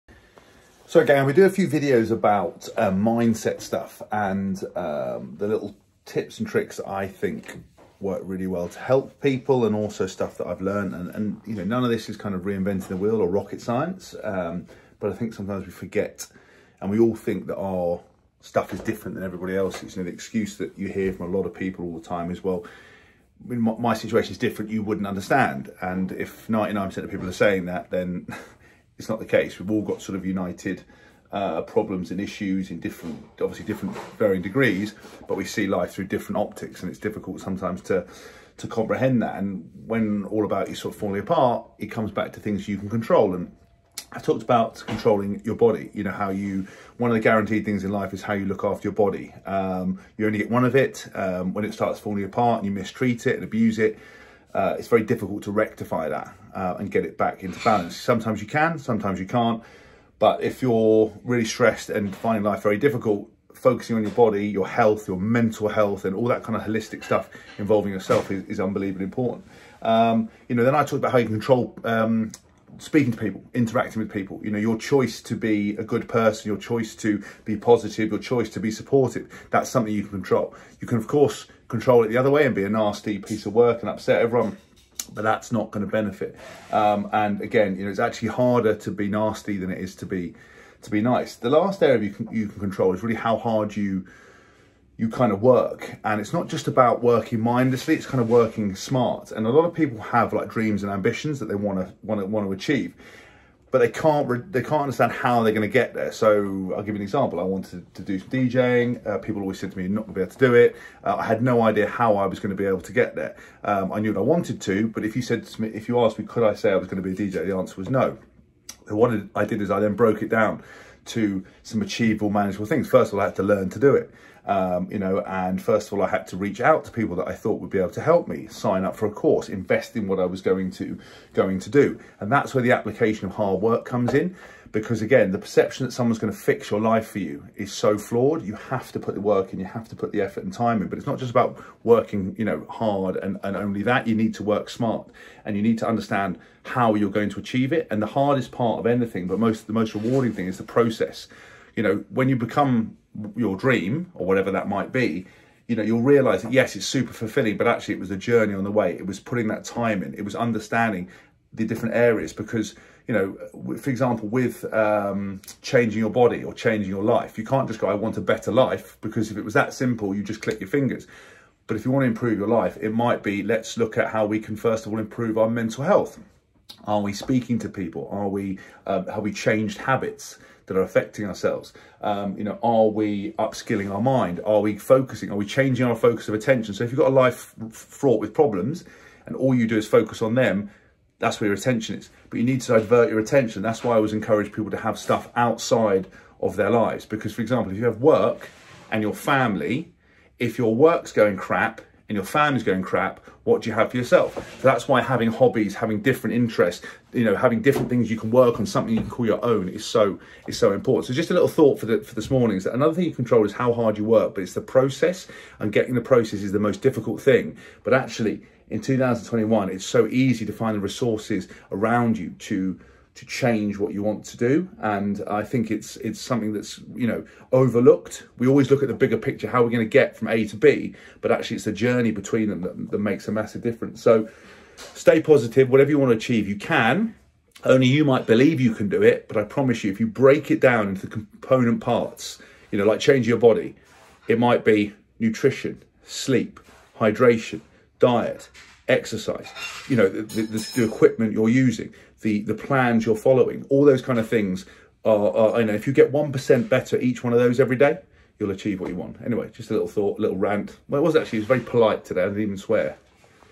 What Can You Control? | James Haskell
I’ve done a few of these on what you can control. One of the fundamentals is how hard you work. Its your choice as to how you apply yourself.

The process of change is hard, how you are supposed to do it is hard. The effort of staying in the game is up to you